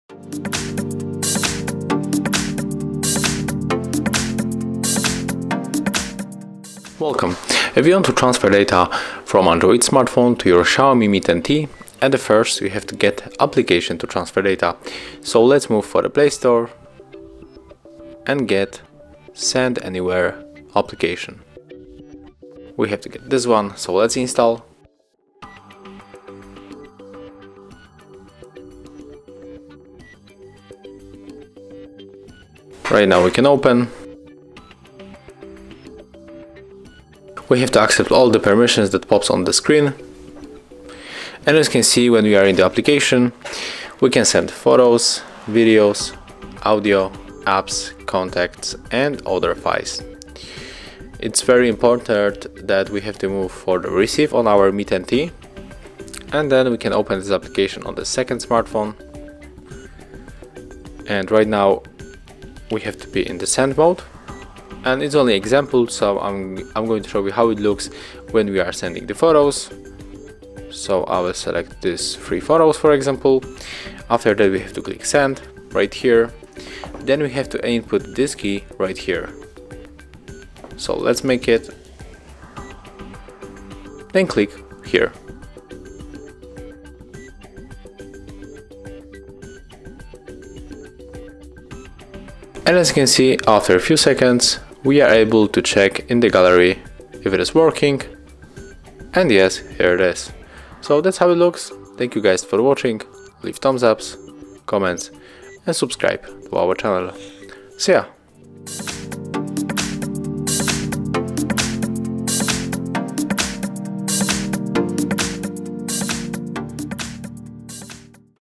Transfer Data to Xiaomi Mi 10T from other Android - Send Anywhere App
If you want to easily transfer all data from your old Android phone to Xiaomi Mi 10T, the Send Anywhere application will help you a lot, it is free and you can easily download it from the Google Play Store.

How to Transfer Data from Android Phone to Xiaomi Mi 10T? How to Move Data from Android Phone to Xiaomi Mi 10T?
How to Move Files from Android Phone to Xiaomi Mi 10T?